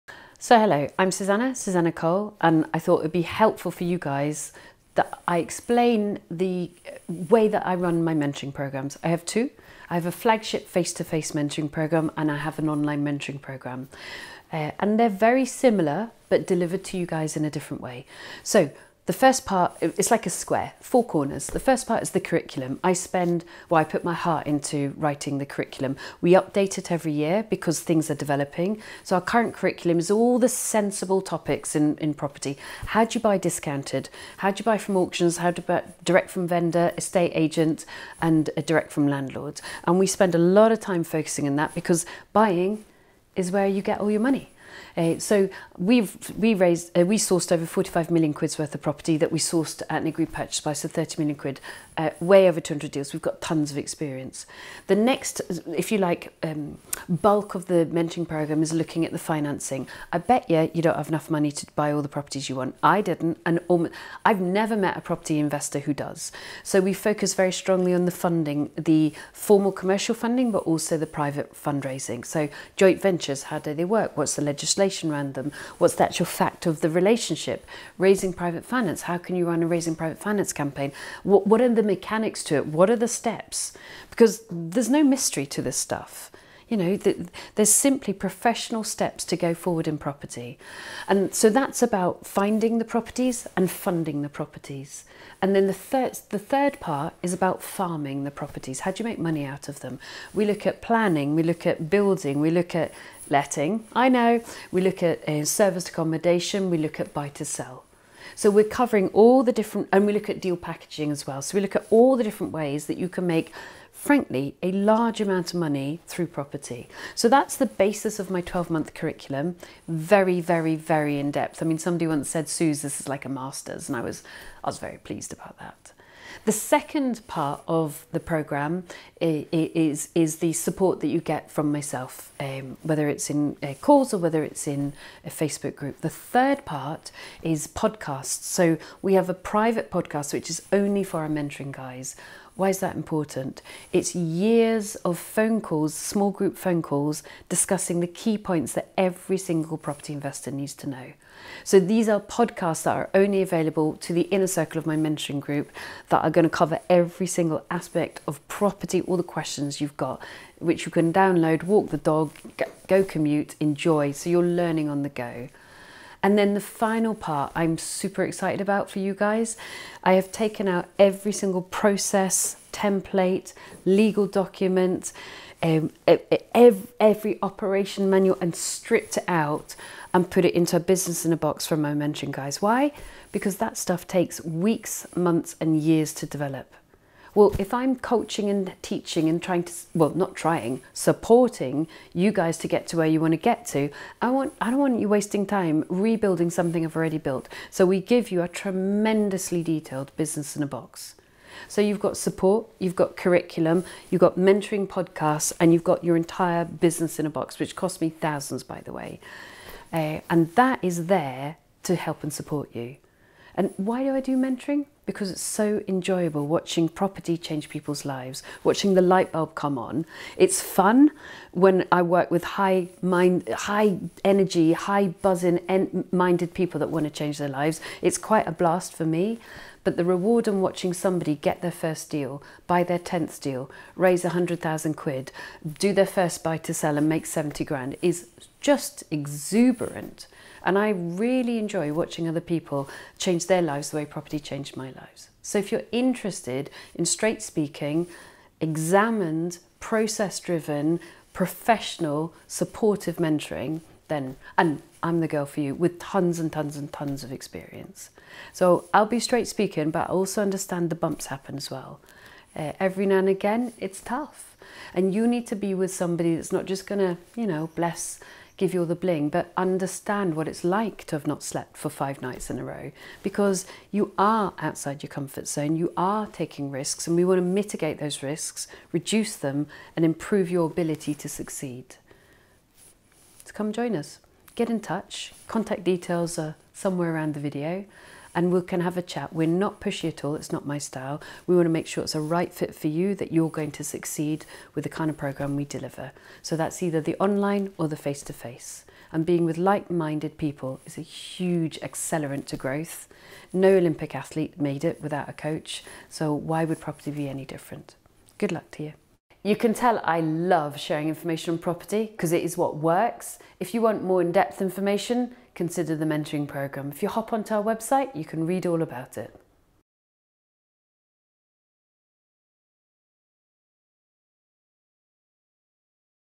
How does Property Mentoring Work?
Info for you on the important Property Mentoring Programme that I run, running through the curriculum, and the rationale behind the structure of the well laid out high quality training.

Good luck to you in property